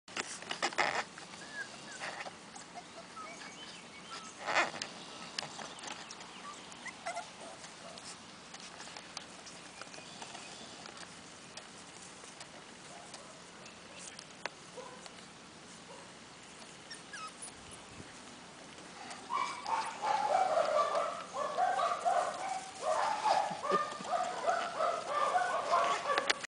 Jelly X Hobie Litter - 5 weeks, 1 day - Part 4
First time in the yard.  Grass is dry because the irrigation pump is broken and we've had no rain in over a month.